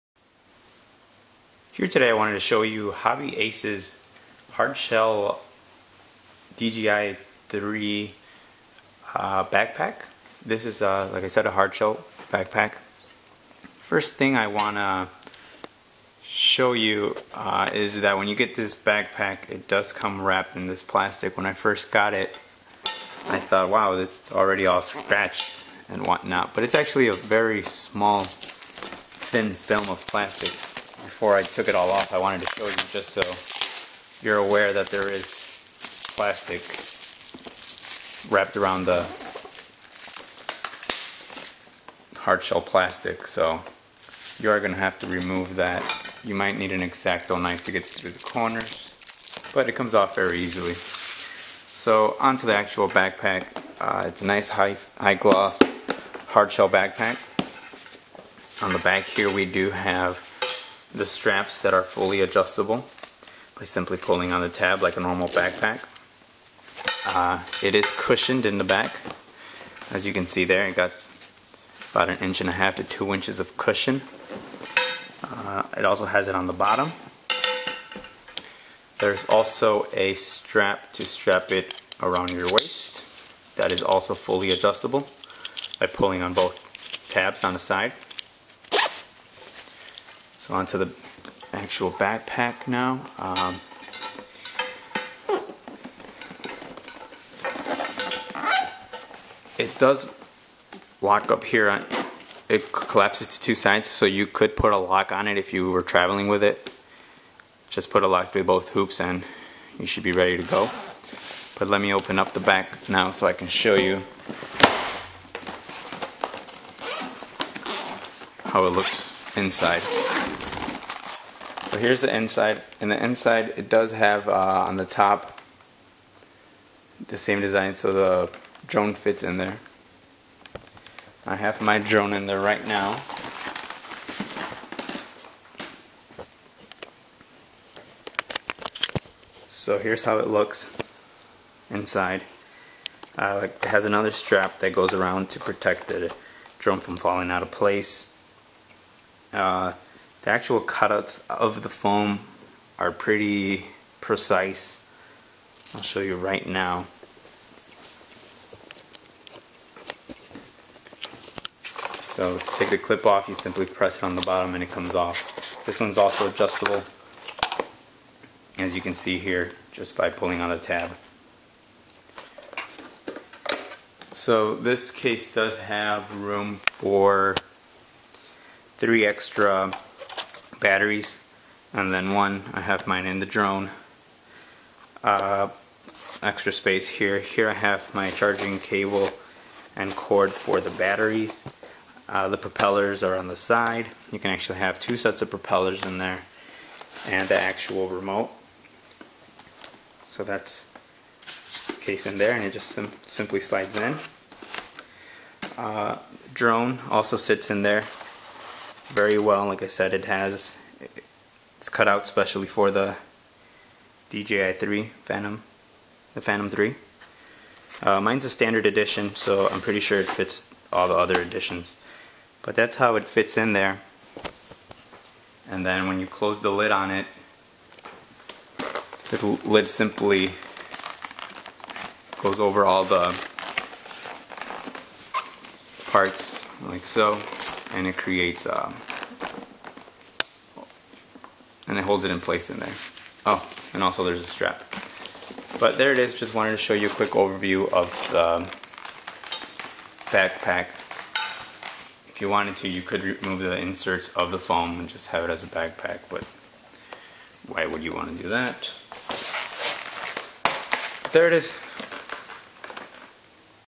Hobby-Ace Backpack Hardshell Case Bag Turtle Shell Waterproof for DJI Phantom 3 Overview
Product description:

For Phantom 3 Advanced and Phantom 3 Professional (water resistant)
DJI exclusive brand new compact design with high-quality abrasion-resistant hard shell
Features pre-cut EVA foam to organize and protect your DJI Phantom 3 quadcopter and additional accessories
Capacity: DJI Phantom 3 x 1 DJI Phantom 3 remote controller x 1 DJI Phantom 3 Intelligent Flight Batteries x 3 DJI Phantom 3 propellers x 4 DJI Phantom series Battery Charger x 1
More DJI product is provided by us in Amazon.Bring your eyes into the sky and racing like a free bird, enjoy the ultimate first person view experience.